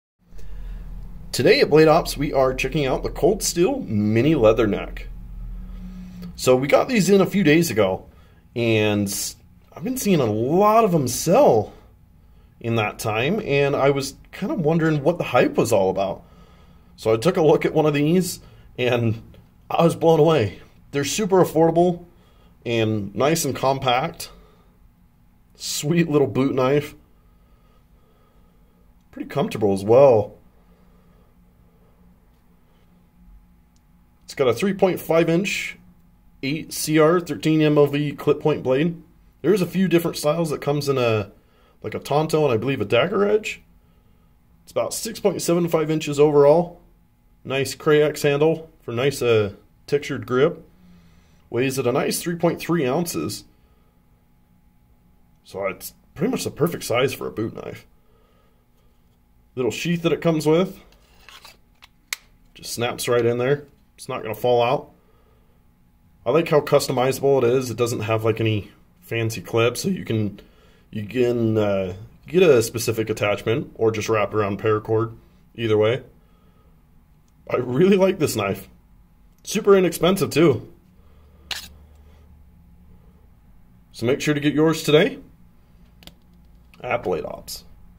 Cold Steel Mini Leatherneck
The perfect little boot knife for an affordable price!

3.5" 18Cr13Mov Clip Point Blade
Secure-Ex Sheath
3.3 oz